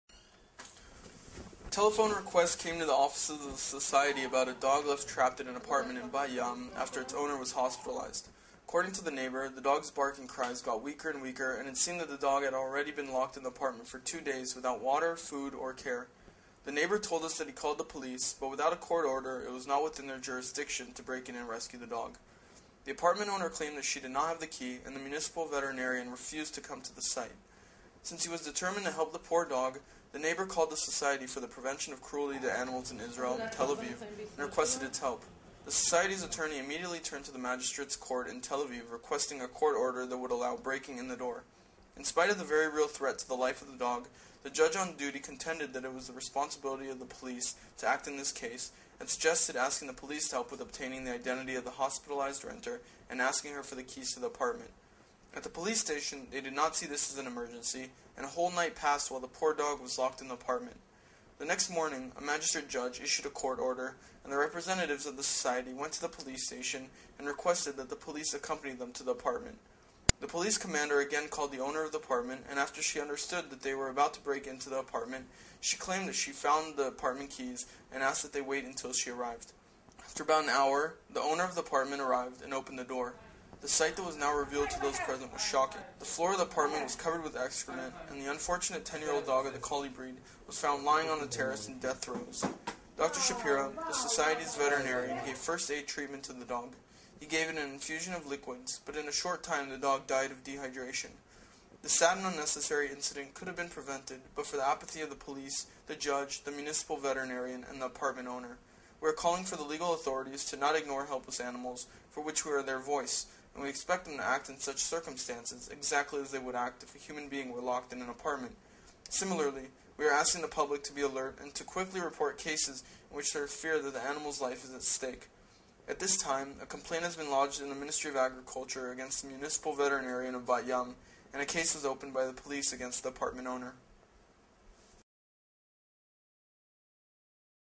A Kafkaesque Situation
Indifference and "red tape" on the part of the legal authorities brought about the death of a dog that was left in a house when its owner was hospitalized.